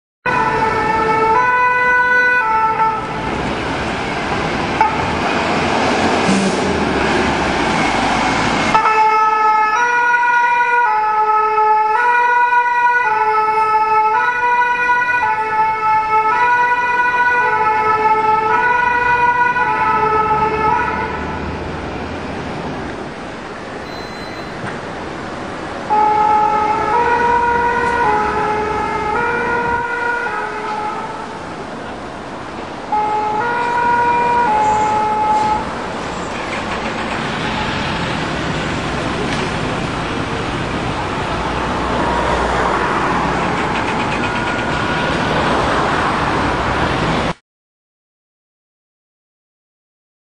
PS 103 BSPP CS Vieux-Colombier
en urgence sur le boulevard St Germain - by Dirk Steinhardt - Paris, Ile-de-France, France - 06.2005